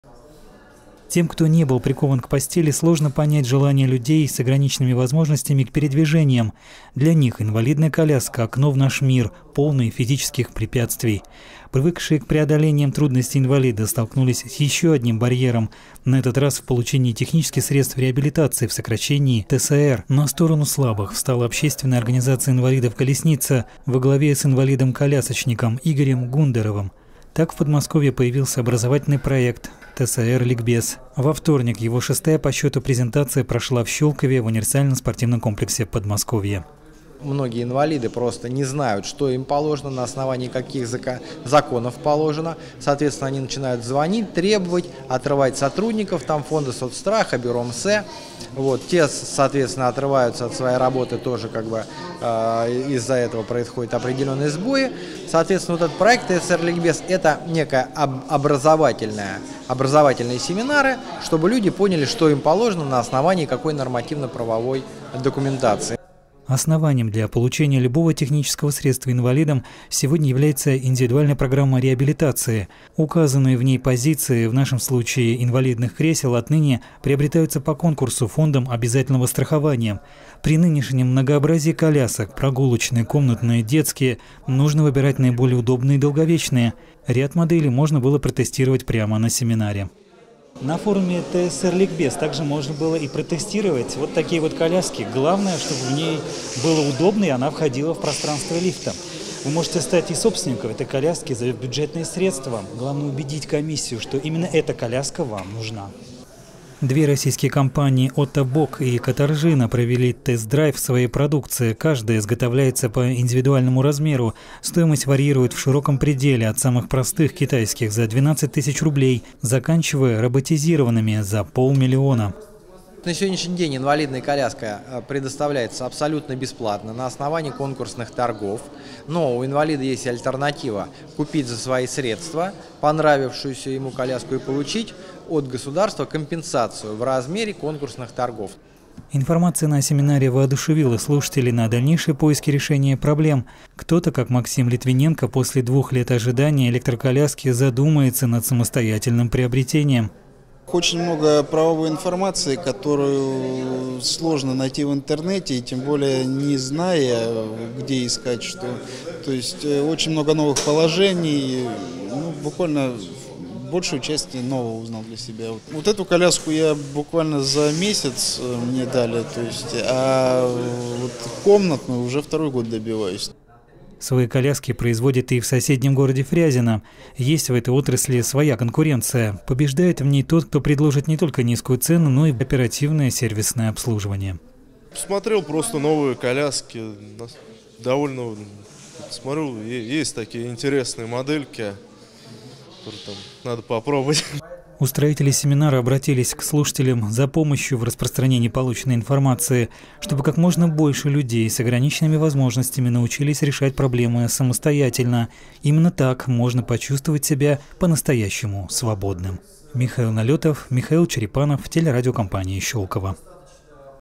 Образовательный проект по освоению технических средств реабилитации презентовали в Щелково
Выход есть. Образовательный проект по освоению технических средств реабилитации "ТСР-Ликбез" презентовали в Щелково. Люди с проблемами опорно-двигательного аппарата узнали, как получить новую инвалидную коляску с минимумом затрат для своего бюджета. Прямо на месте они протестировали современные образцы средств передвижения. Подробности узнаем в следующем сюжете.